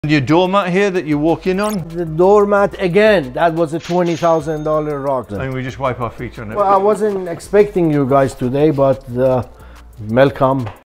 $20,000 DOOR MAT
We always love visiting Freddy's storage unit. Who uses a $20,000 rug as a door mat?